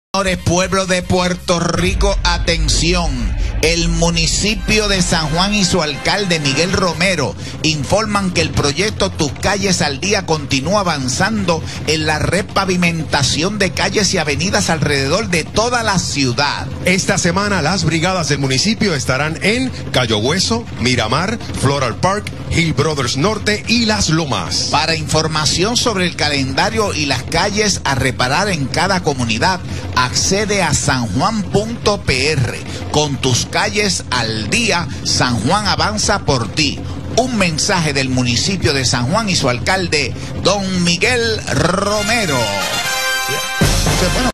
la comay mensaje municipio de San Juan
"tus calles al día" continúa avanzando en la repavimentación de calls y avenidas en san juan